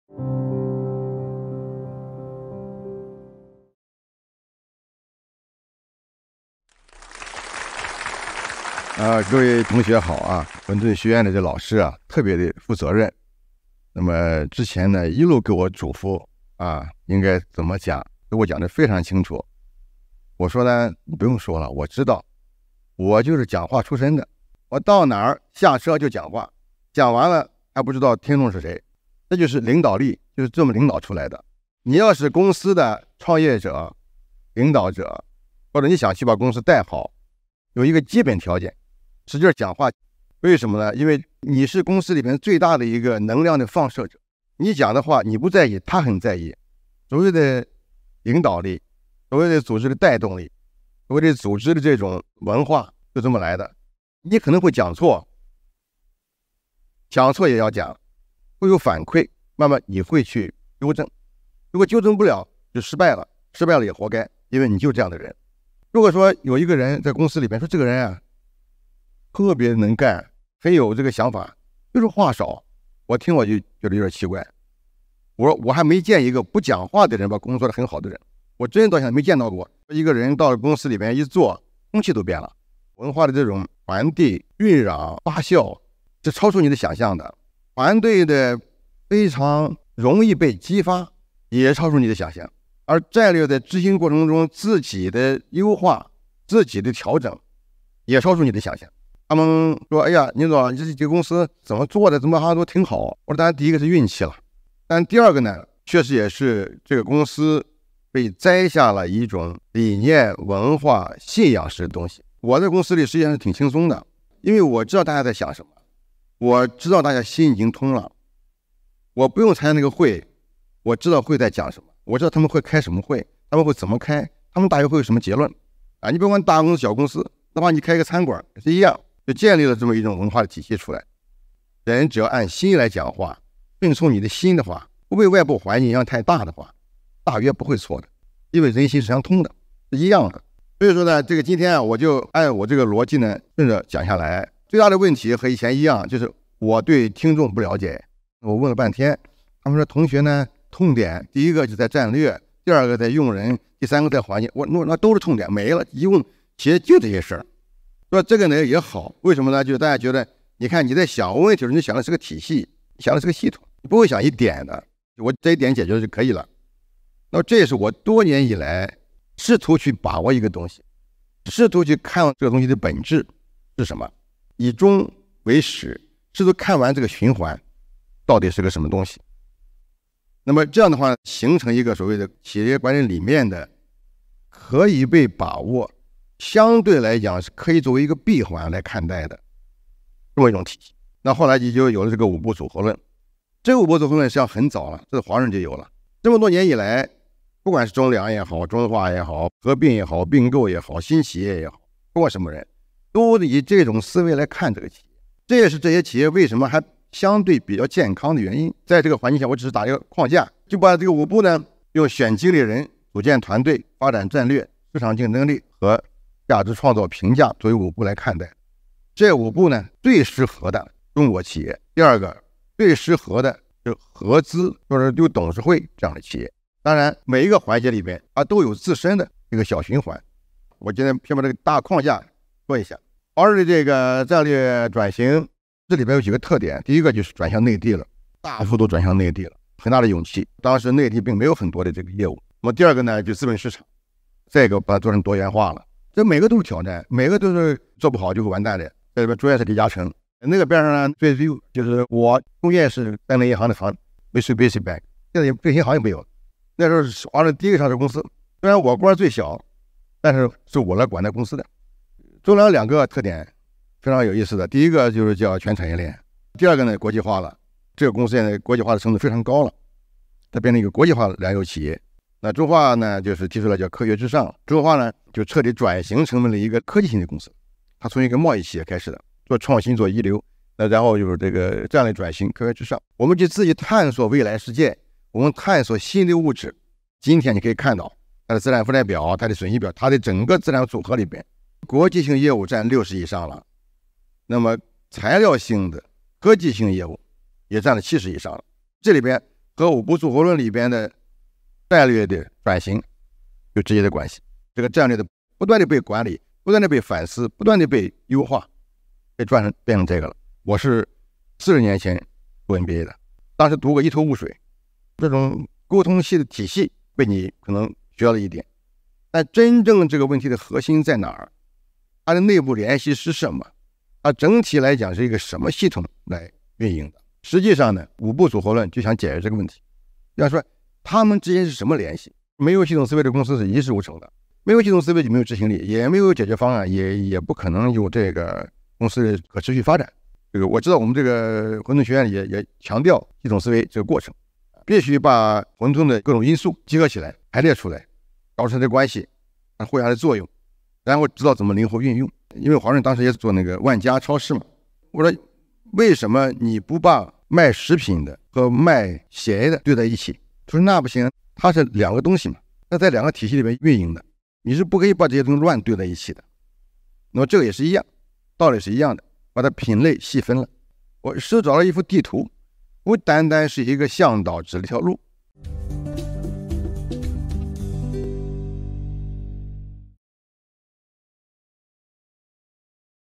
五步组合论：更适合中国商业的管理方法论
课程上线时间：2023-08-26

宁高宁
宁高宁
中国中化原董事长、党组书记

“没有系统思维的公司是一事无成的。”
“企业文化就是团队散了会之后讨论的事情”
“在选人上有‘人感’，在带团队上要‘感人’，这就是领导力的核心。”
“在真正的管理学里，人就是全部。”

宁高宁老师，是极具影响力的中国商界领袖，曾任华润集团副董事长、总经理，中粮集团董事长，中化集团董事长，中国化工董事长，以及两化合并后中国中化董事长，是迄今为止执掌过4家“世界500强企业”的中国企业家。他主持了华润变革、中粮转型、“两化”重组，为中国的经济进步和企业发展做出了巨大的贡献。

在35年的职业生涯和管理实战中，宁老师坚持系统观念，运用系统思维，进行系统性变革，创新系统性管理理论，形成了“五步组合论”、“6S管理”、“战略十步法”、“价值管理四要素”等实践性很强的管理思想和理论方法，既推动企业实现了跨越式发展，又为中国管理思想的理论建设和实践探索提供了新的理论范式和研究路径。

外界给了这位专治大型央企疑难杂症的大掌门诸多标签，比如：“红色摩根”、“救火队长”、“中国韦尔奇”等等。但他最独特且宝贵的标签或许是：他是最有企业家精神，最不像“领导干部”的央企领导人之一，也是最善于把乱牌、杂牌打成大牌和王牌的中国企业家之一。

8月26日，这位“最具有企业家精神”的企业家将来到混沌，分享他在商场戎马半生积累的企业决策智慧，为大家带来构建企业战略管理的系统思考。宁老师强调，战略是一种思维方式，是积极创造价值的过程，是团队价值观和思维方式的体现。在全球新变局下，企业战略如何定位并落地执行？如何选人用人，打造企业市场竞争力？所有的内容干货，尽在《五步组合论：全球新变局下的企业战略》中！